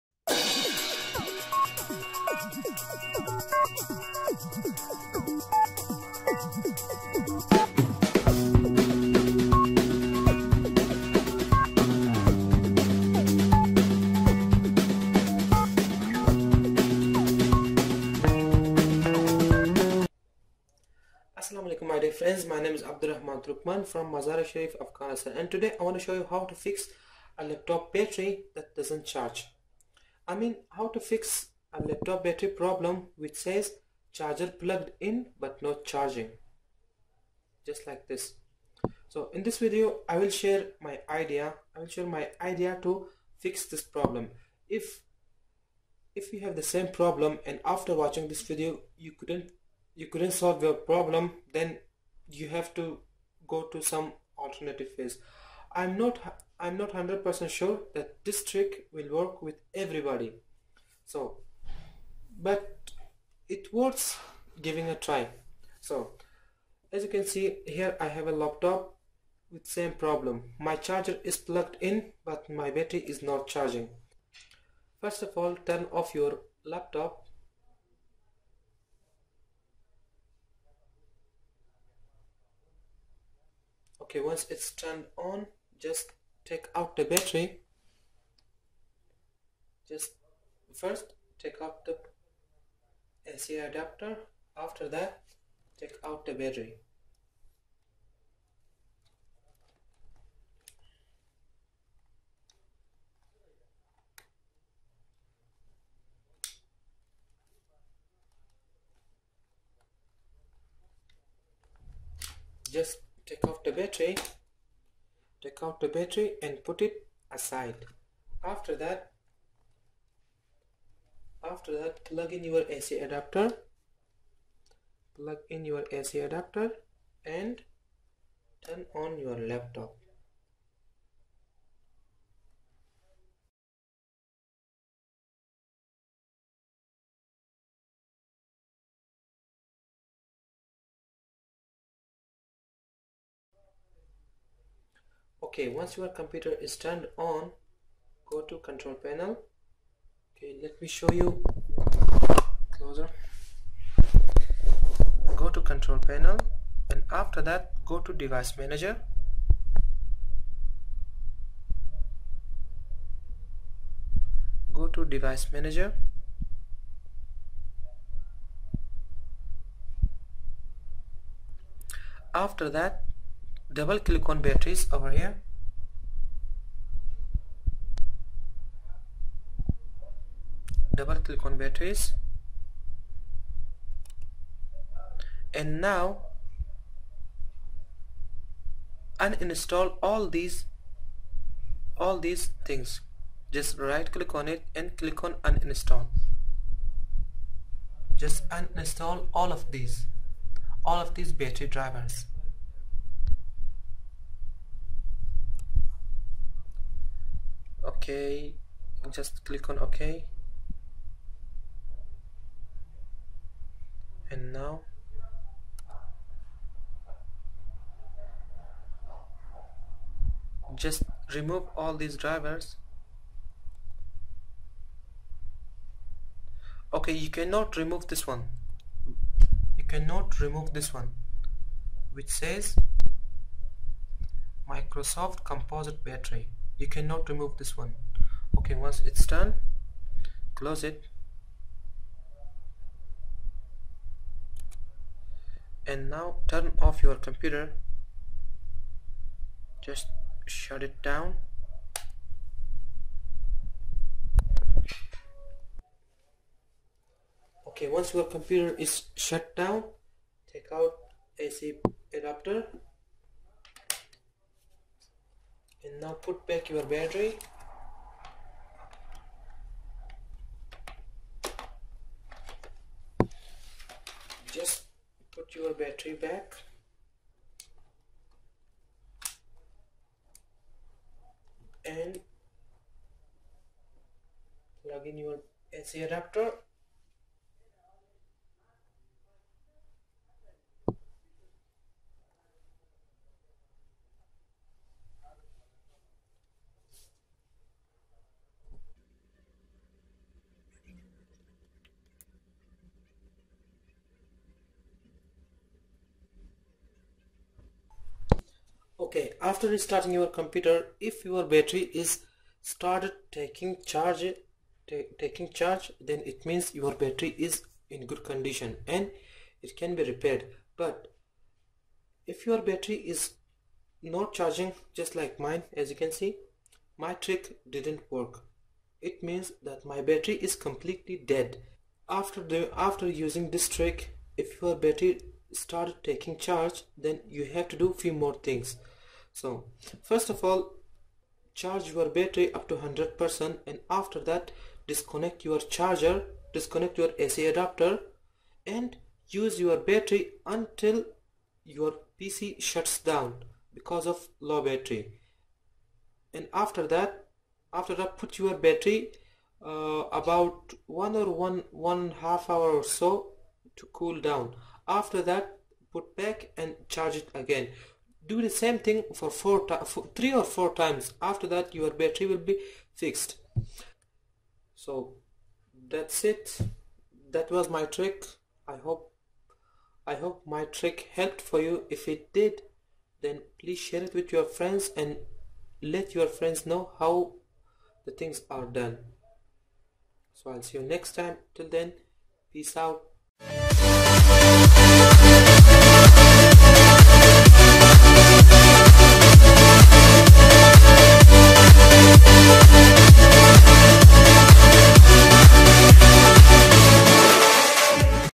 How To Fix Laptop Battery That Doesn't Charge
how to fix laptop battery problem that says "plugged in but not charging"
in this video i will show you a trick to fix the battery that doesn't hold charge.

SUGGESTED VIDEOS.